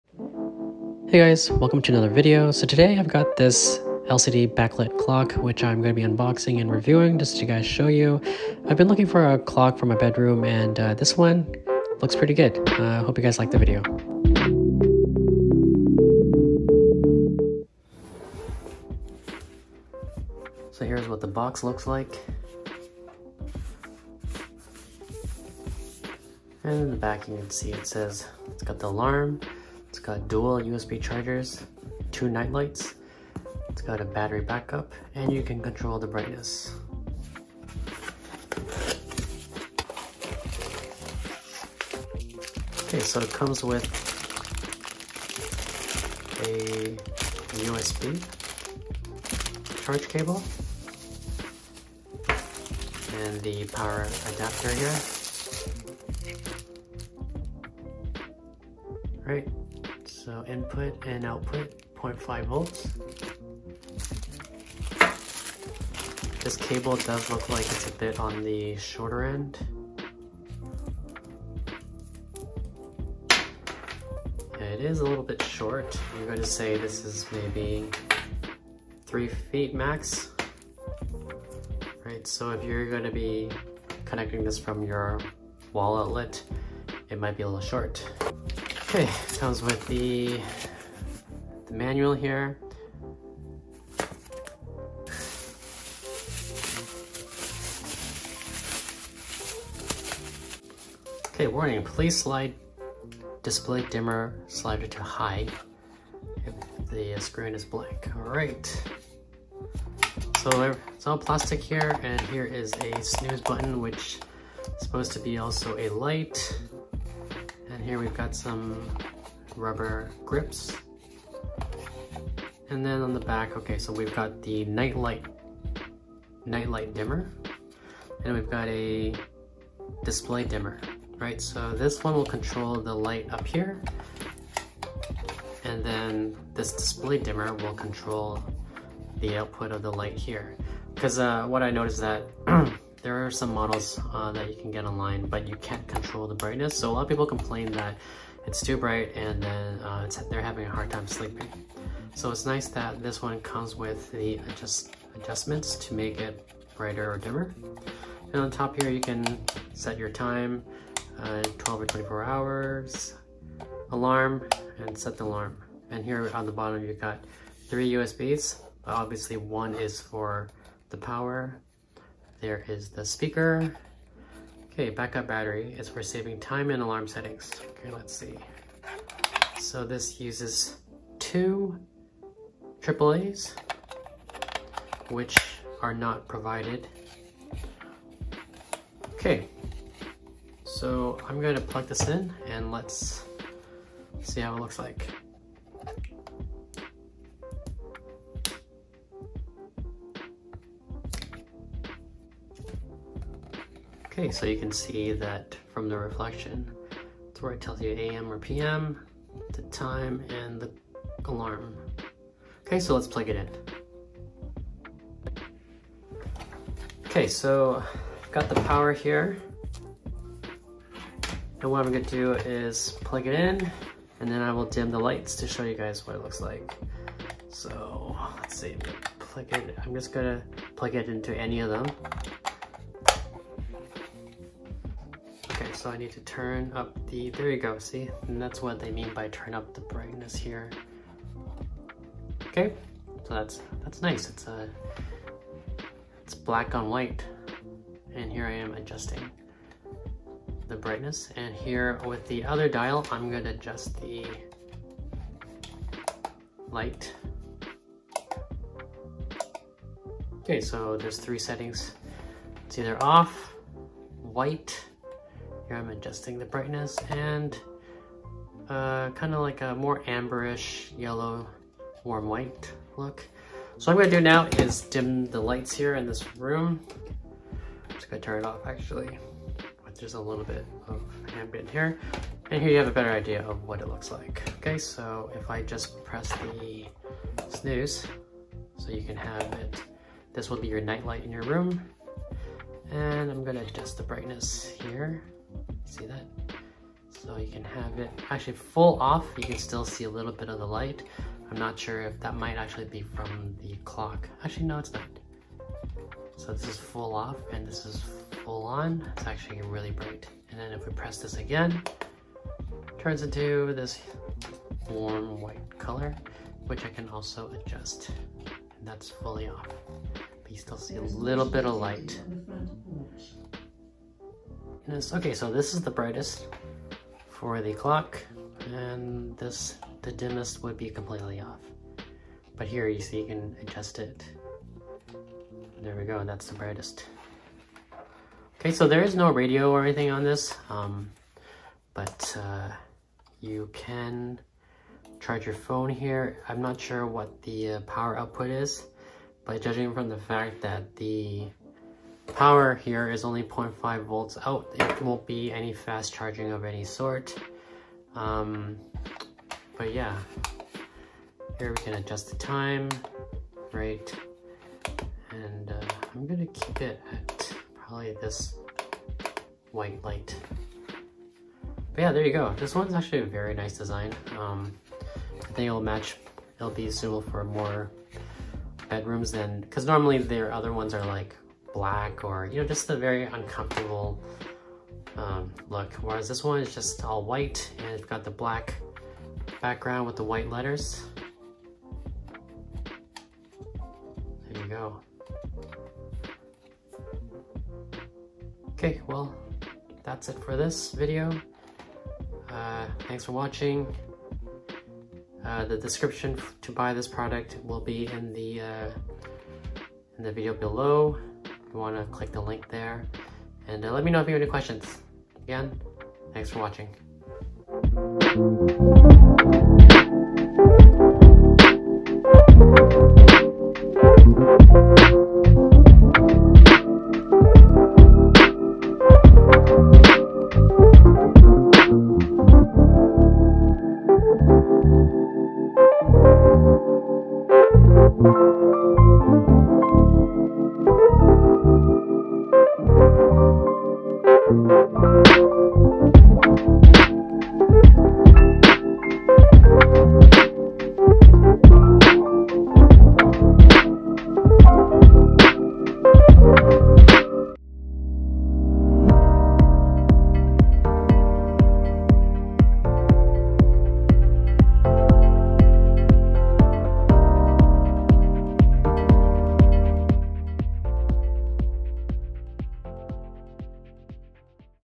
Discover the Best Modern Digital LED Clock: A Must-Have for Your Home!
Amazon US
Amazon Canada

This is the Housbay Digital Alarm Clock with USB.

This is the perfect desk clock for you if you need to wake up at night to do anything. The alarm goes off at the precise time you had set and wakes you up gently with a gradually rising sound. The nightlight can be of great help if your kids does not like sleeping in the dark.With dual USB ports and nightlight equipped, this clock will be your best night-time companion.
This bed alarm clock gives you two color options when it comes to nightlight. You can choose to have a white or orange nightlight color. You can dim the nightlight down to 10% or you can have the nightlight as high as 100%. This provides a wide range of nightlight brightness for you to choose from.

Enjoy the unboxing video.   Here I show you what is in the box and the accessories it comes with to give you an idea of what to expect.